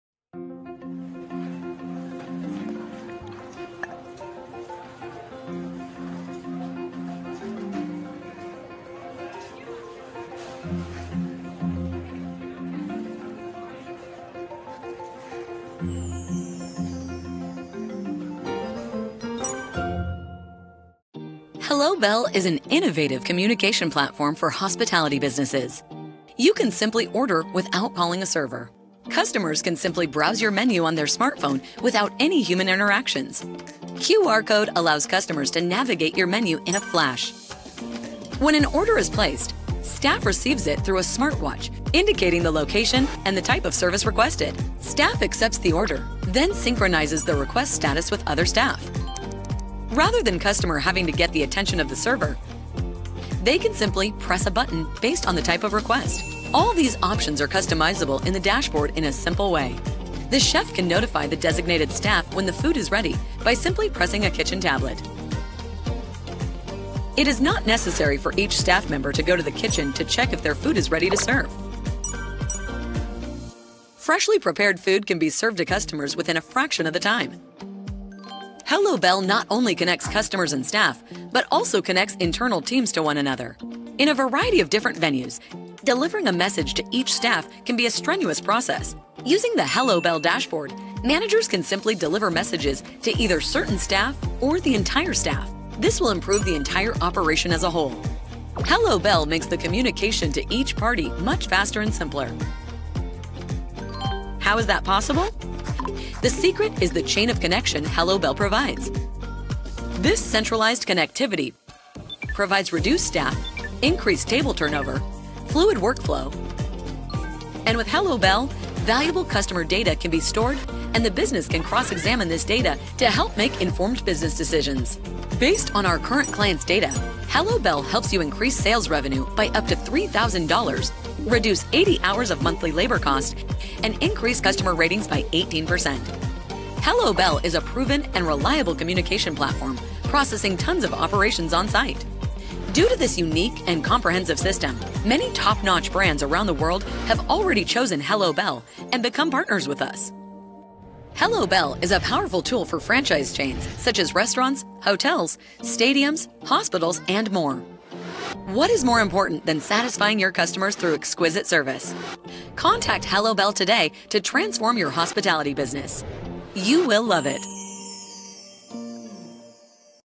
헬로벨 소개 영상 (한글자막)
호스피탈리티에서 사용하는 커뮤니케이션 플랫폼 헬로벨의 소개 영상입니다.
헬로벨의 기능과 차별점, 그리고 고객이 어떠한 혜택을 받을 수 있는지 확인해 보세요~~